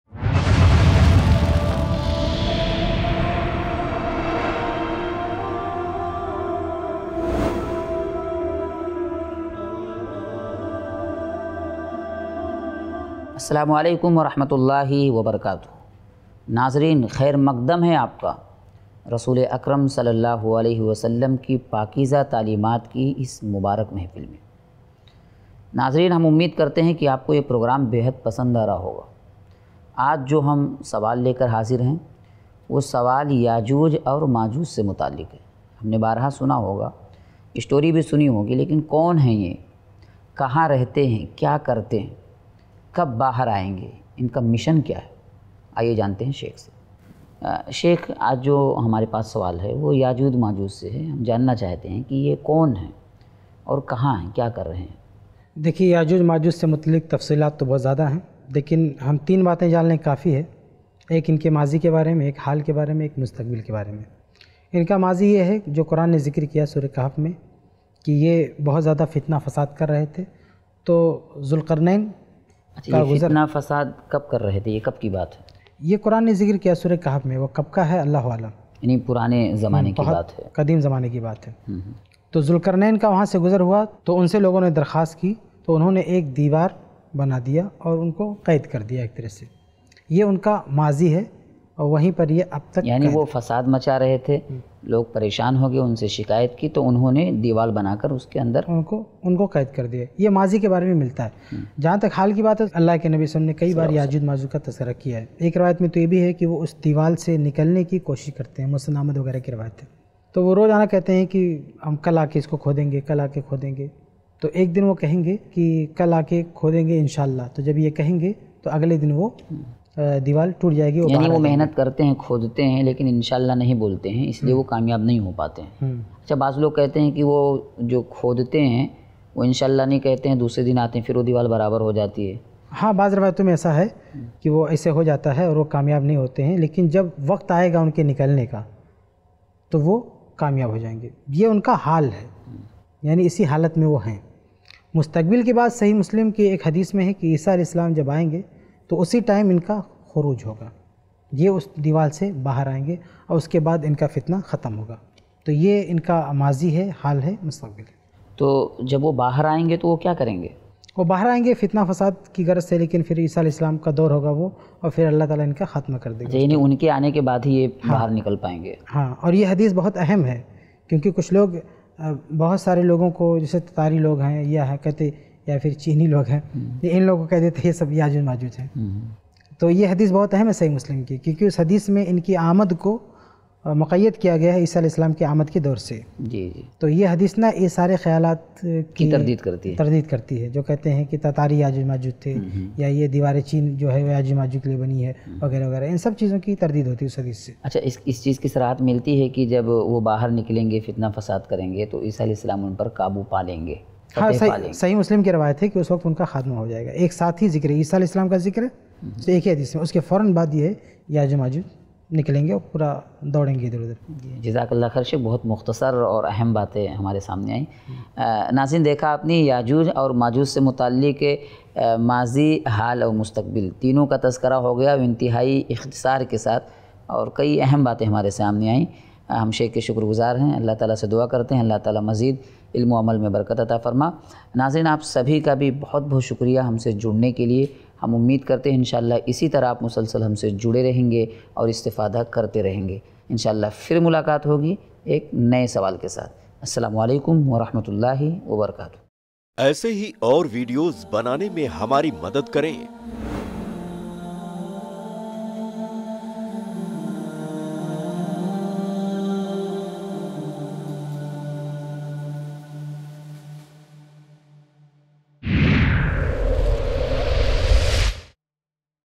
Yajooj Majooj Kaun Hain? Kab Aayenge ? Kahan Rehte Hain? | Rubaru 58 | Shaikh Kifayatullah Sanabili
Yajooj Majooj Kaun Hain? Kab Aayenge ? Kahan Rehte Hain? | Rubaru 58 | Shaikh Kifayatullah Sanabili and Shaikh Shahfahad Sanabili | iPlus TV